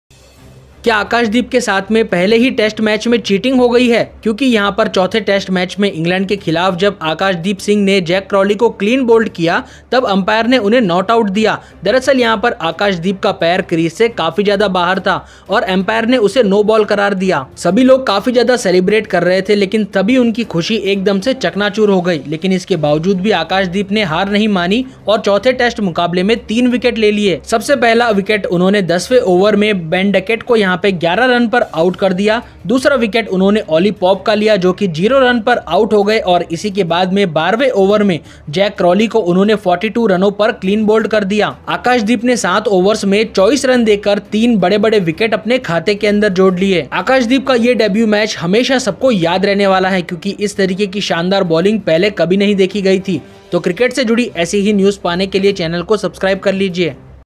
Umpire Did Cheating With Akash deep In Ind vs Eng 4th test match | akash deep cricket news facts
akash deep cricket news today | akash deep bowling today | akash deep wickets today | akash deep news